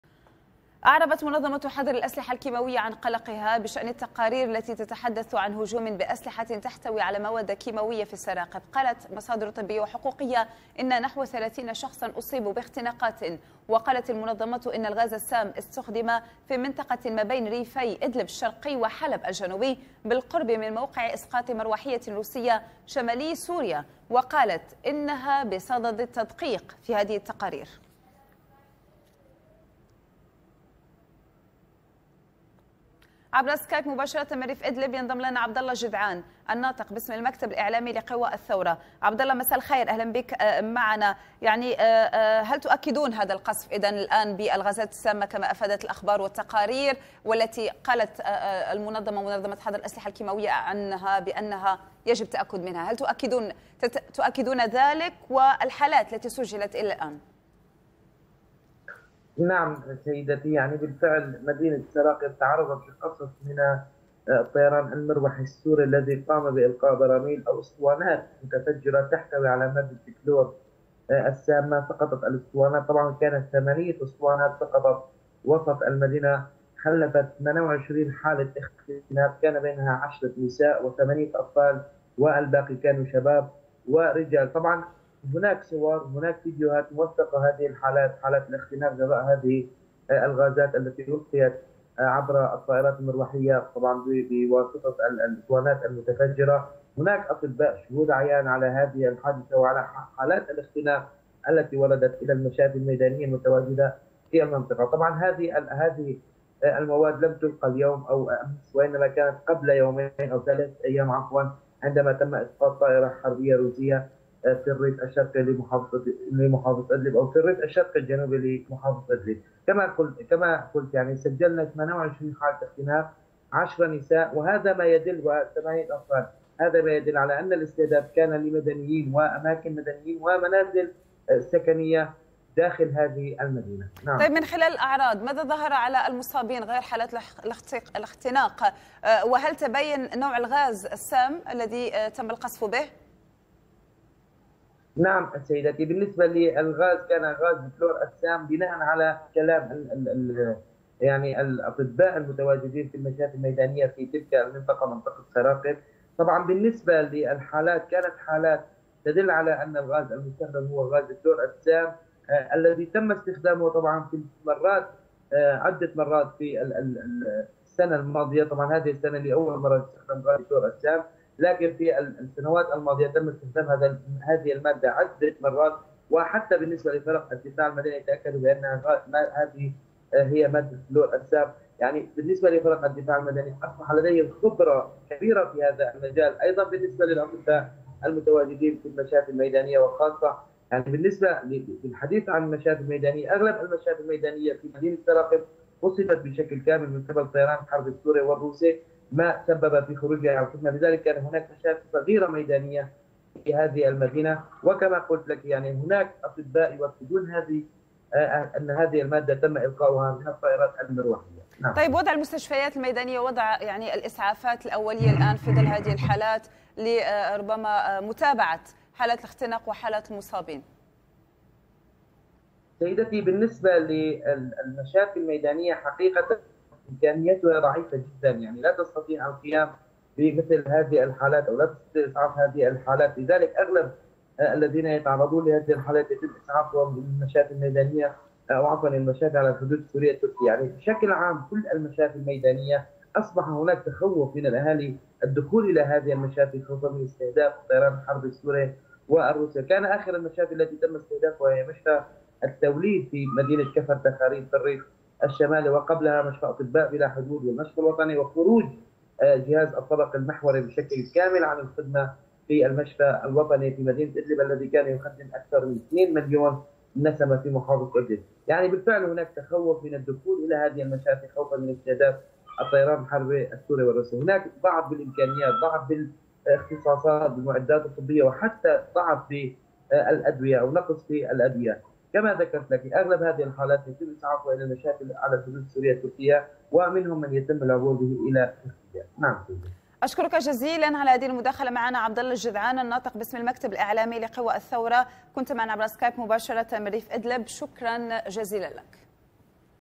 قلق أممي من استخدام أسلحة كيمياوية بمعارك إدلب السورية
شاهد آيضا:
قيادي بعثي يفضح تاريخ آل الأسد!؟
أعربت منظمة حظر الأسلحة الكيمياوية عن قلقها بشأن التقارير التي تتحدث عن هجوم بأسلحة تحتوي على "مواد كيمياوية" في سراقب قرب حلب. وقالت مصادر طبية وحقوقية إن نحو ثلاثين شخصا أصيبوا باختناقات.
وقالت المنظمة إن الغاز السام استخدم في منطقة بين ريفي إدلب الشرقي وحلب الجنوبي بالقرب من موقع إسقاط مروحية روسية شمال سوريا وقالت إنها بصدد التدقيق في هذه التقارير.
ومن جانبها، لفتت واشنطن إلى أنها تدرس في صحة هذه المزاعم التي وصفتها بالخطيرة للغاية" إن ثبتت.

وكان مسعفون وحقوقيون أعلنوا تعرض نحو ثلاثين شخصا لاختناقات إثر إسقاط مروحية عبوات من الغاز السام على سراقب عقب إسقاط الطائرة الروسية.
وفيما اتهمت المعارضة نظام الأسد بالهجوم الكيمياوي، أكدت موسكو أنها أبلغت واشنطن بوقوع هجوم_كيمياوي لا تعرف طبيعته في حي صلاح الدين بحلب الشرقية.

ويشار إلى أن قوات الأسد قصفت بغاز الكلور السام أرياف إدلب وحماة ودمشق وحلب خلال العامين الماضيين، وخلفت الهجمات عشرات القتلى والمصابين.
وتبنى مجلس الأمن الدولي العام الماضي قراراً يدين استخدام غاز الكلور في سوريا، من دون توجيه الاتهام لأي طرف، وهدد بفرض إجر